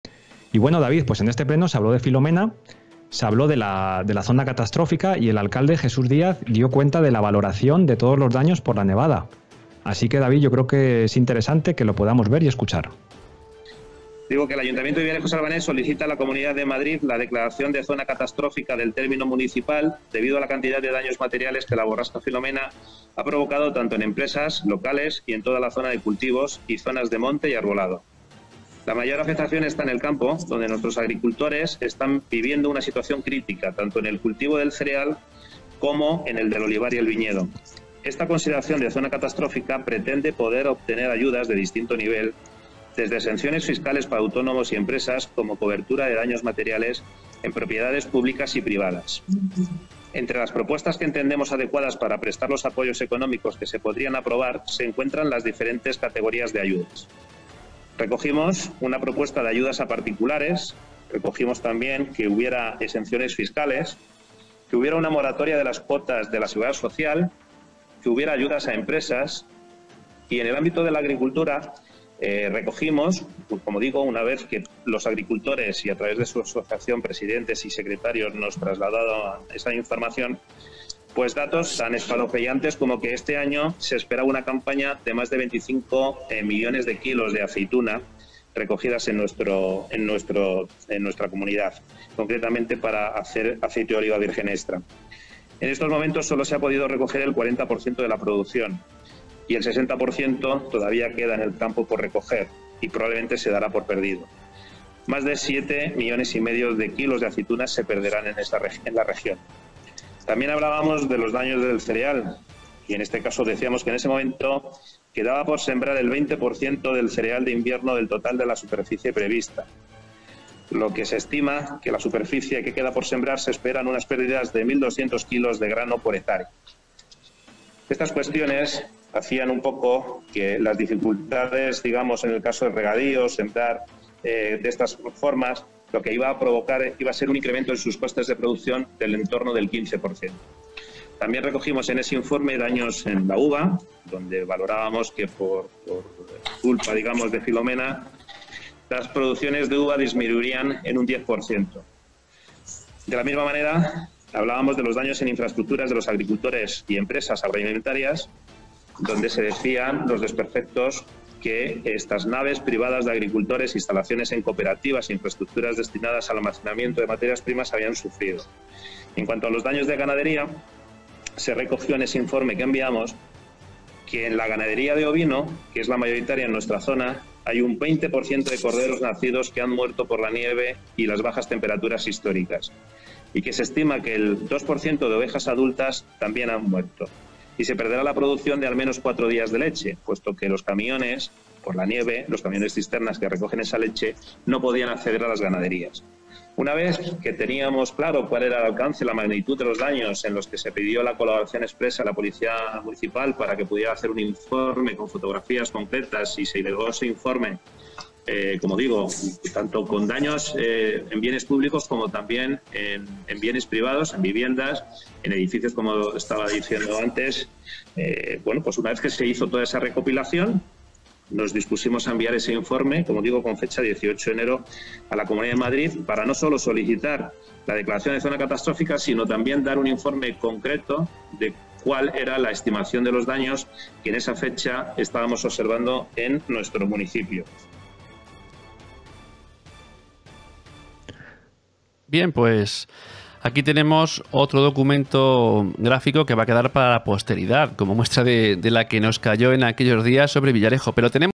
EN0x74-Clips. Daños provocados por la nevada contados por Jesús Díaz en el Pleno de Villarejo
Te ofrecemos la sección donde el alcalde Jesús Díaz Raboso en el Pleno hace un repaso de los daños provocados por la nevada, dando cuenta de la declaración de la zona catastrófica. Este corte se incluyó en el repaso semanal de la actualidad de Villarejo desde el 1 de marzo de 2021 que se emitió en DIRECTO el pasado 8 de marzo para las plataformas de Youtube y Facebook simultáneamente.